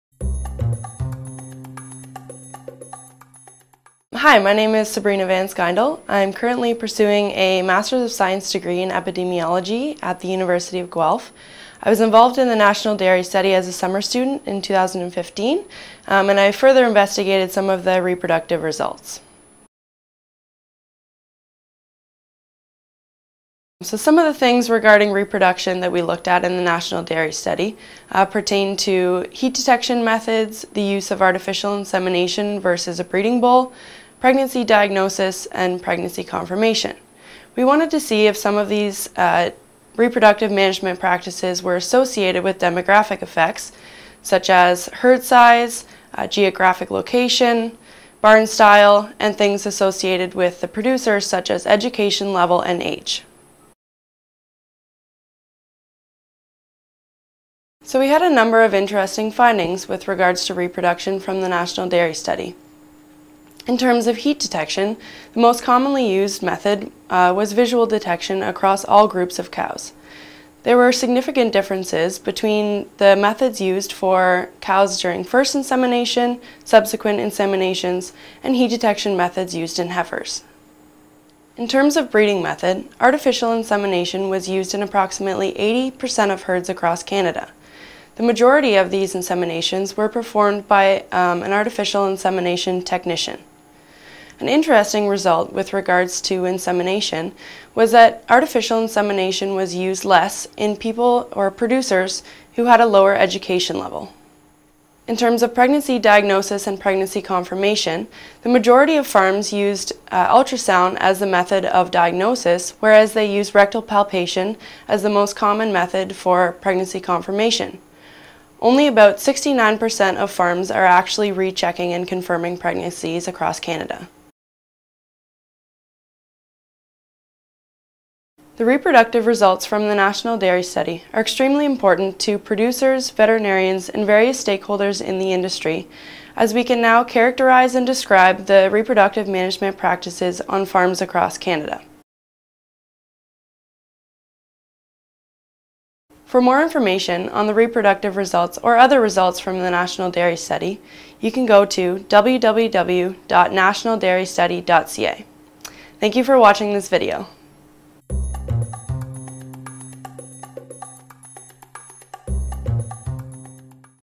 Sabrina Van Schyndel Research Vlog
Watch as Sabrina Van Schyndel describes her research on reproduction practices in the Canadian dairy industry, as part of the National Dairy Study. for more information!